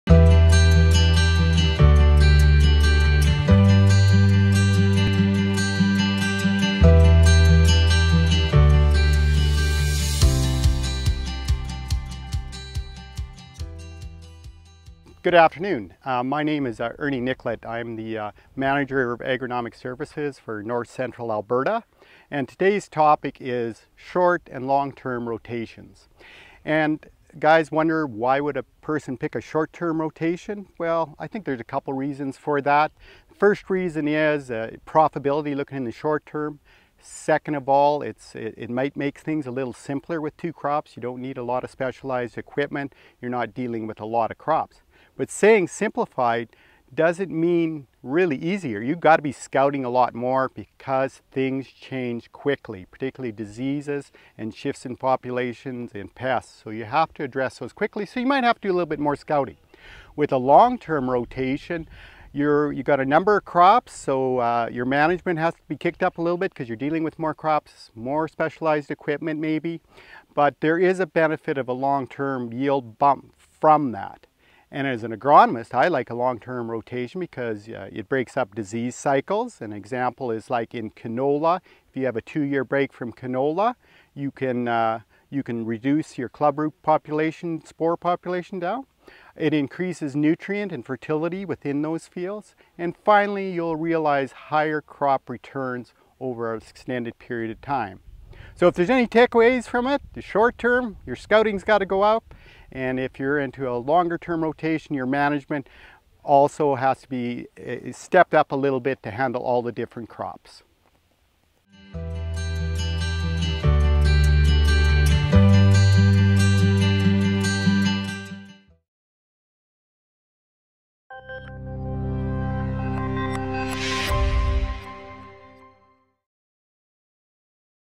Agronomy Minute - Short and Long Term Crop Rotations
Nutrien Ag Solutions' Manager of Agronomic Services Ernie Nycholat talks about crop rotations and compares the advantages and disadvantages of short and long term crop rotations.